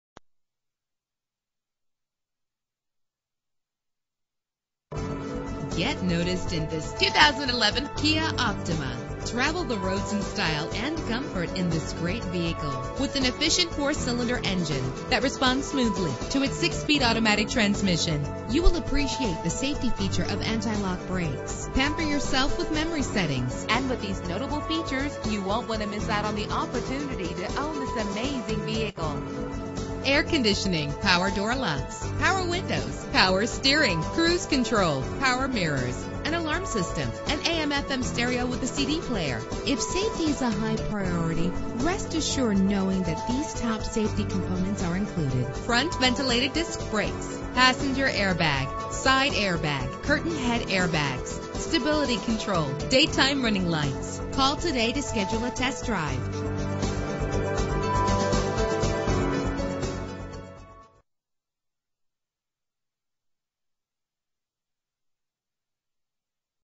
2011 Kia Optima - Gainesville GA
Year: 2011
Make: Kia
Model: Optima
Trim: LX
Engine: 2.4 liter inline 4 cylinder 16 valve
Transmission: 6-Speed Automatic
Color: Snow White Pearl
Mileage: 9
Address:
2801 Browns Bridge Rd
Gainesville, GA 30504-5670